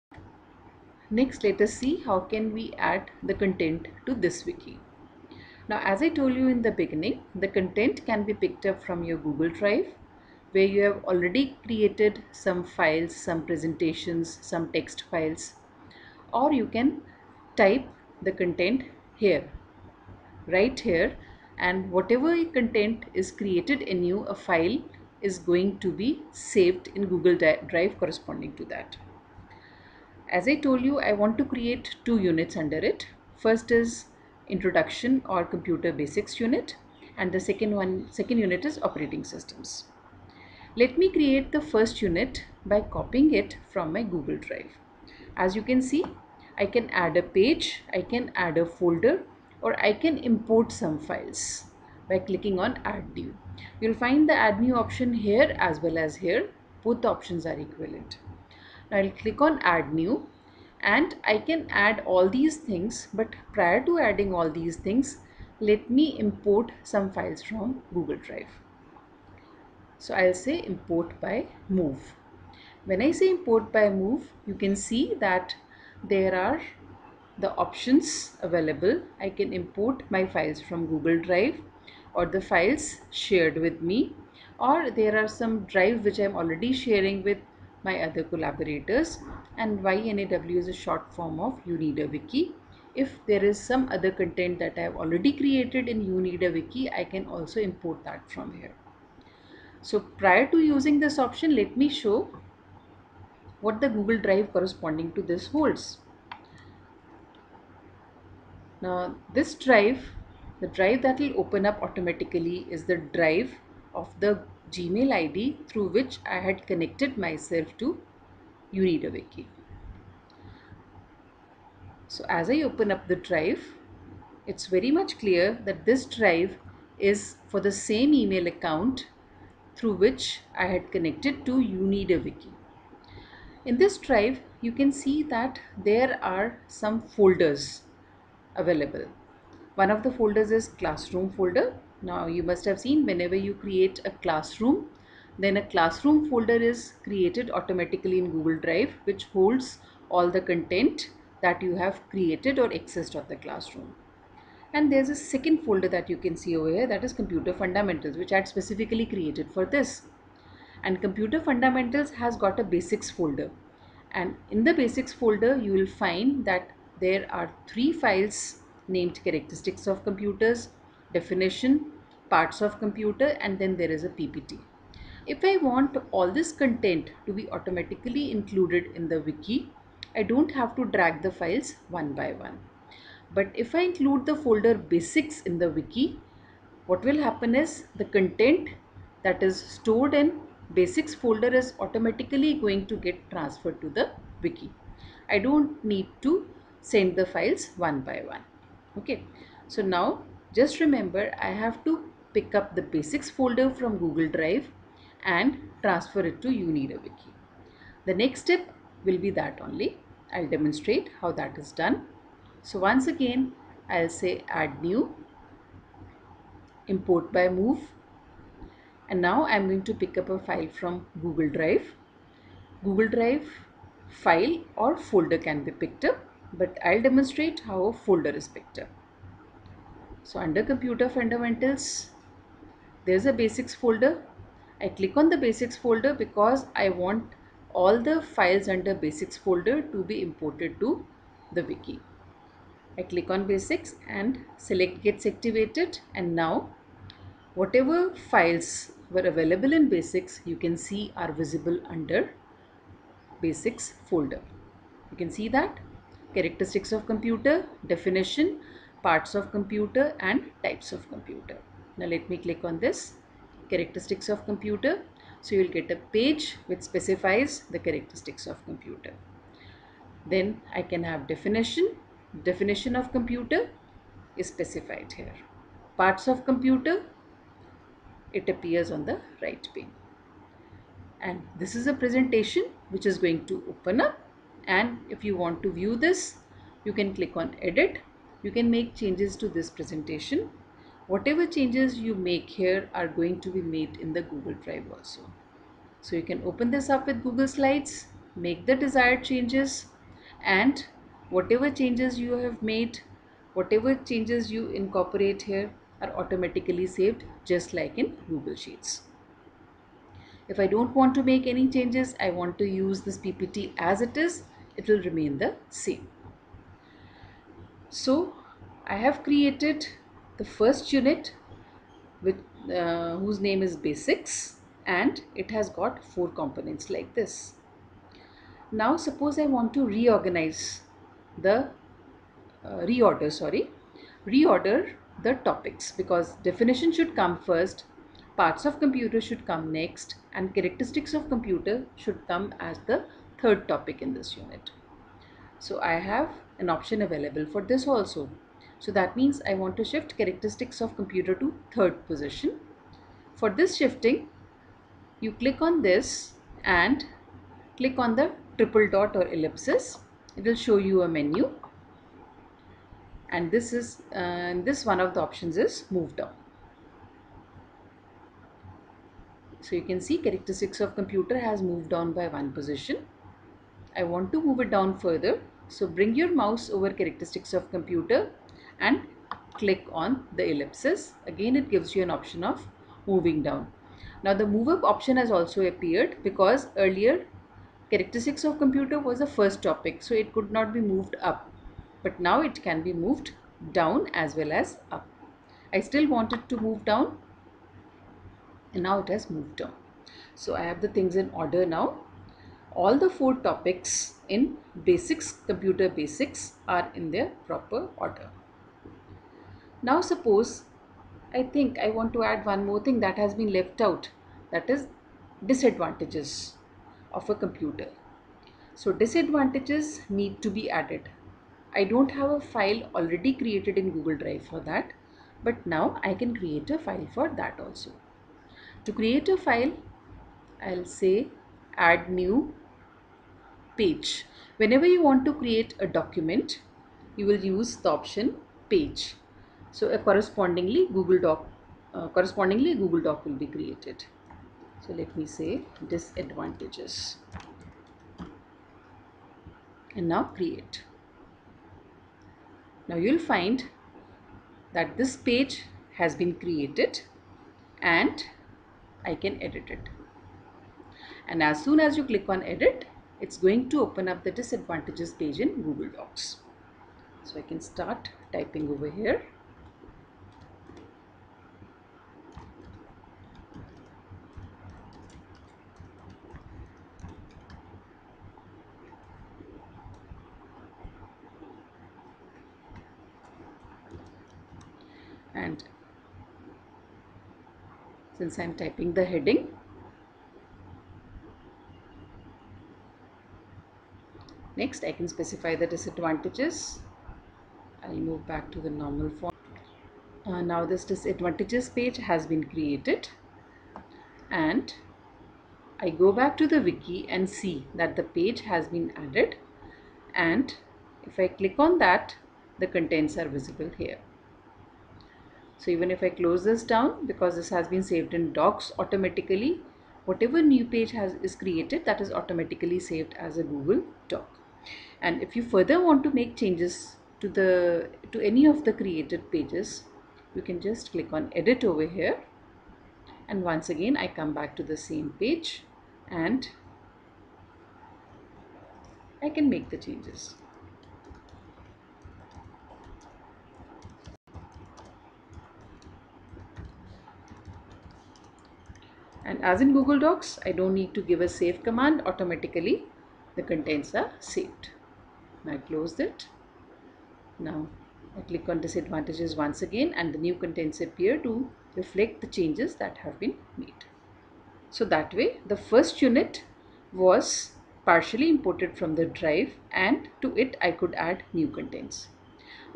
You Need A Wiki - Part 2
This video demonstrates the use of "You Need a  Wiki" App for creating wiki pages. Using this app,  the instructor/teacher can create wiki pages of the content for the benefit of students.